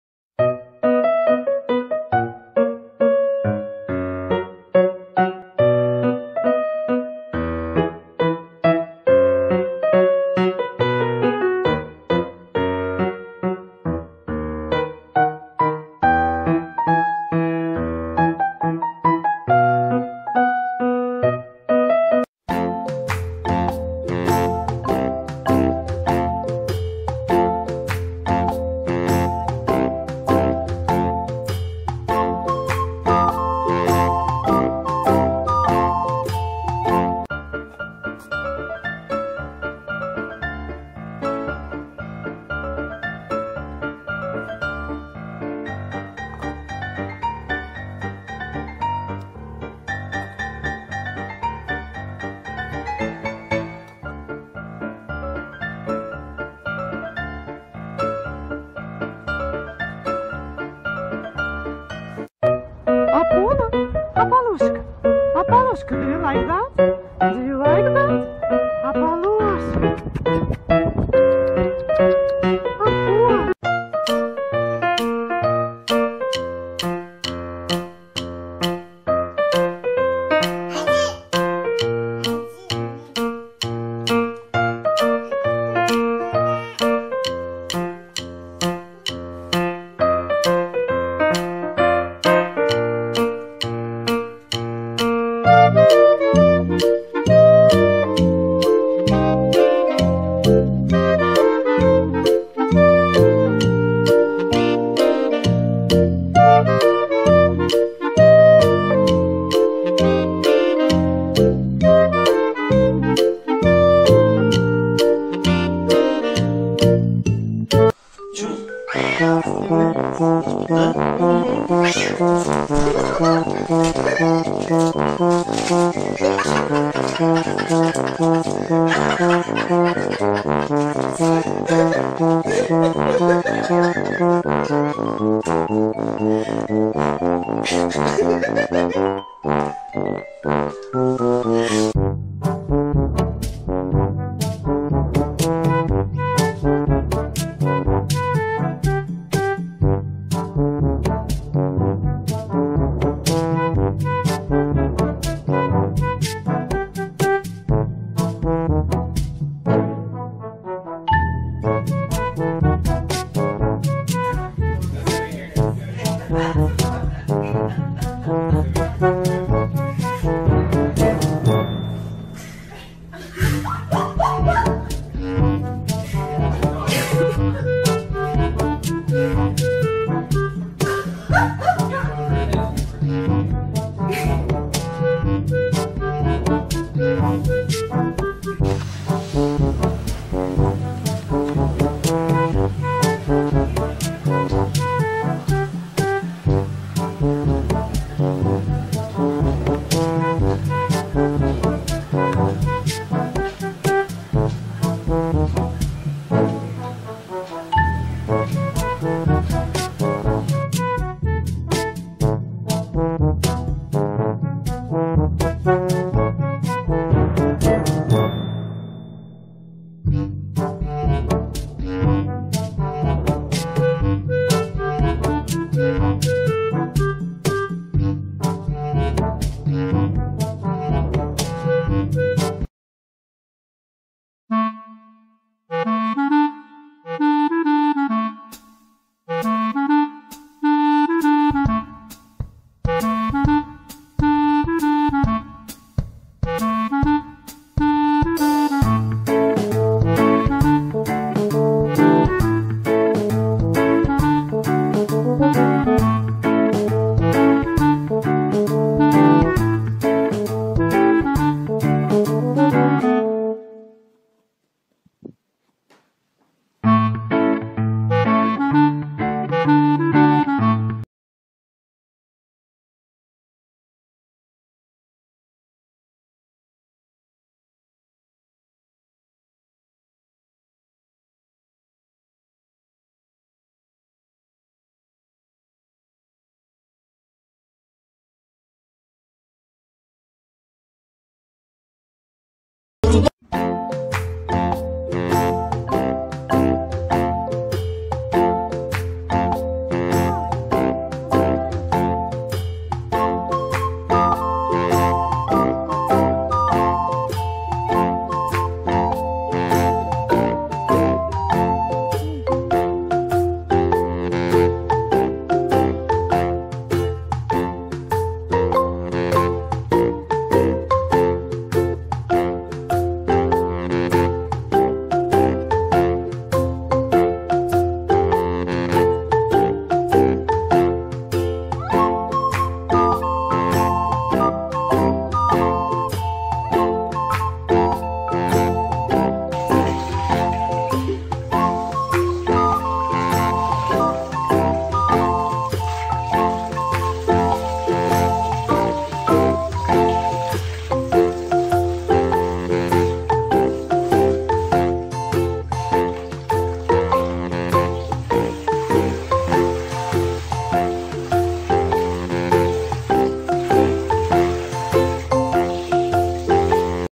Lustige Tier Videos - Lachen garantiert!
Ein Mix aus vielen Tier Videos zum Totlachen, viel Spaß beim Lachen gerade in der momentanen Zeit eine willkommene Abwechslung.

Viel Spaß!